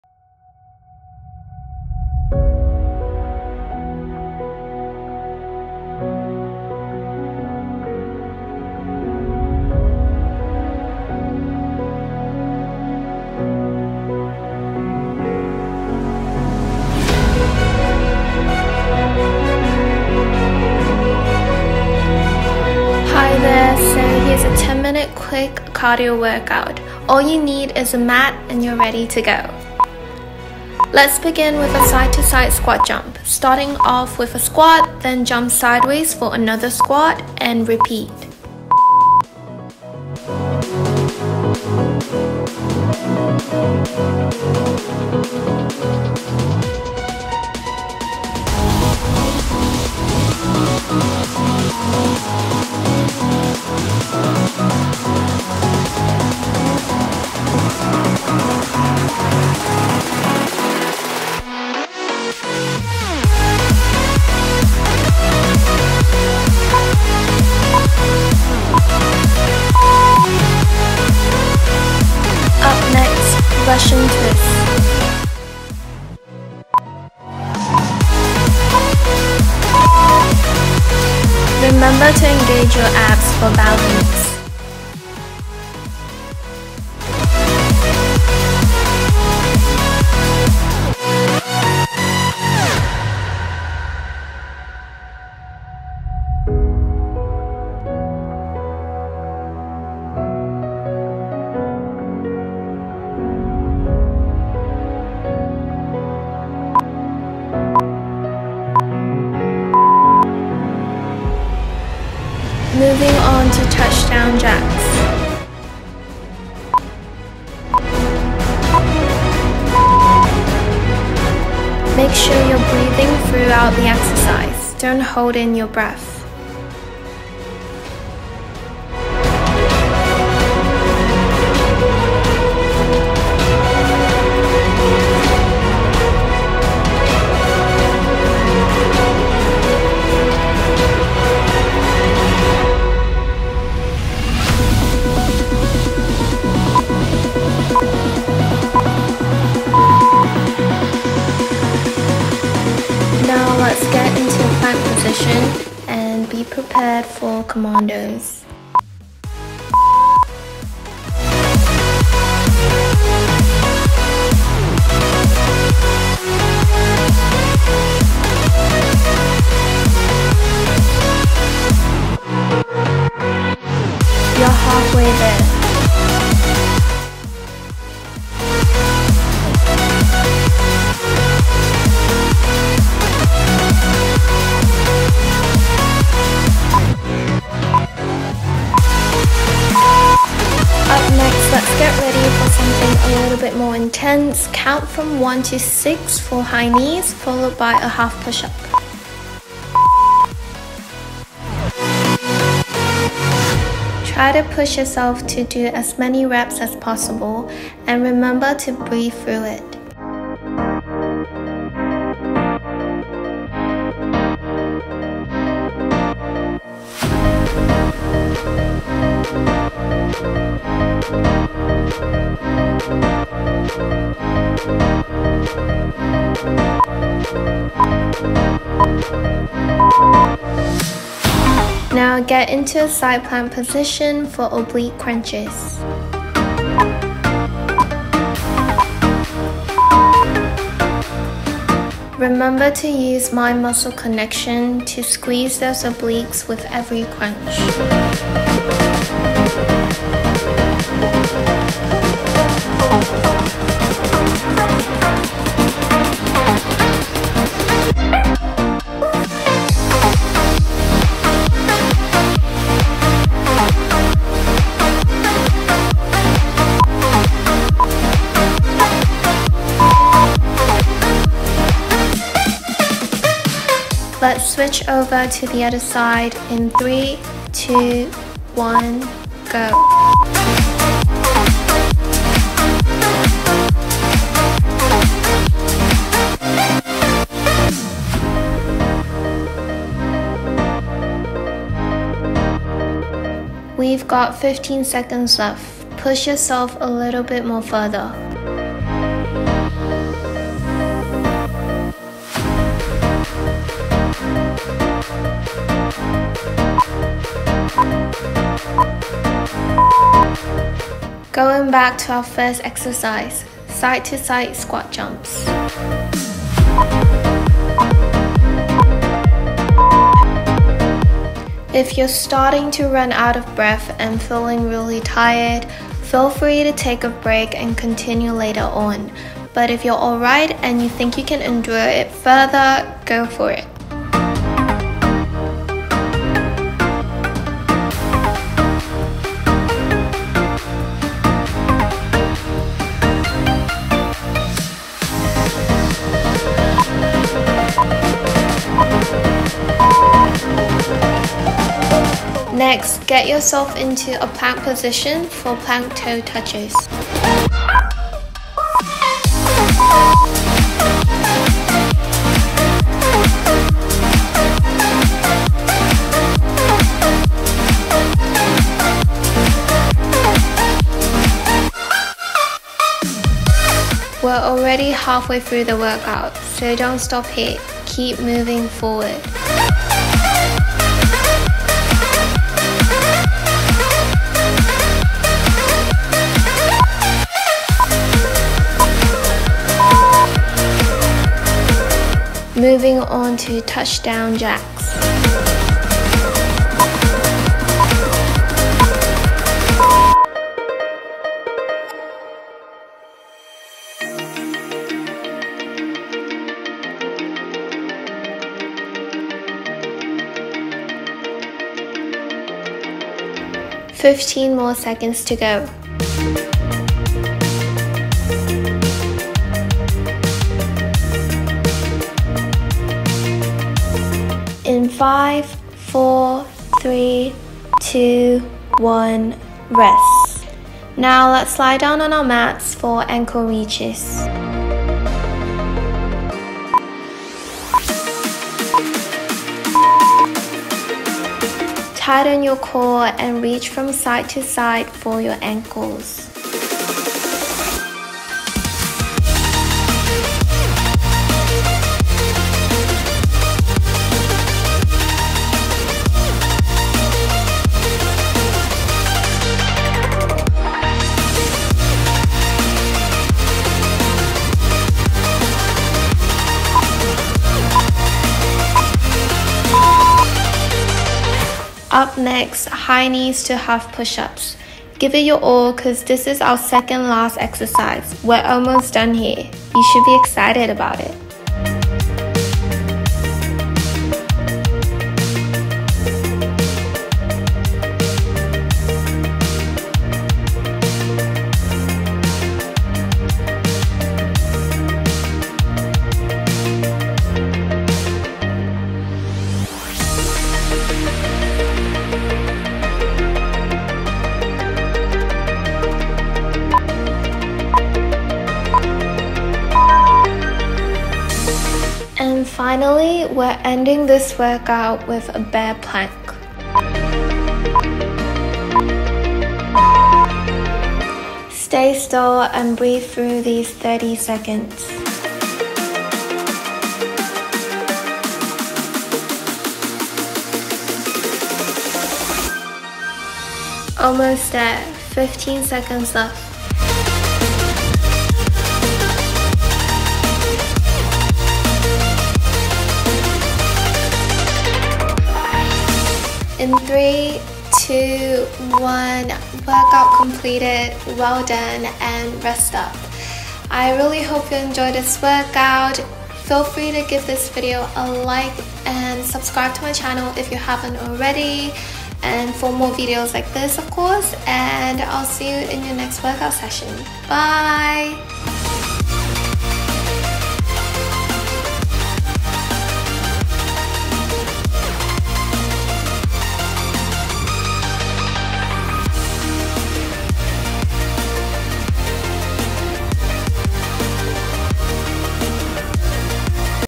10 MIN CARDIO (Calorie Killer) WORKOUT | No Equipment ~ Jacey Yaw
Workout
Hi everyone! Here’s a Fat Burning Cardio Workout for you to challenge yourself! Enjoy~

► WORKOUT TIMING⏱**?** 40s; 10s Rest
► EQUIPMENT? NO
► EXERCISES🧘🏻‍♀️**:**
1. Side to Side Squat Jump
2. Russian Twist
3. Touchdown Jacks
4. Commandos
5. High Knees to Half Push-Ups
6. Oblique Crunches (R)
7. Oblique Crunches (L)
8. Side to Side Squat Jumps
9. Plank Toe Touches
10. Touchdown Jacks
11. Ankle Reaches
12. High Knees to Half Push-Ups
13. Bear Plank

► ALWAYS…
* Have a Well-Balanced Diet 🥗 + 🍕
* Have a Healthy Mindset ☺️
* Workout Consistently 💪🏻
* Be Patient with Yourself 💕

I have made my home workouts into playlist so you don't have to 😄 Check them out!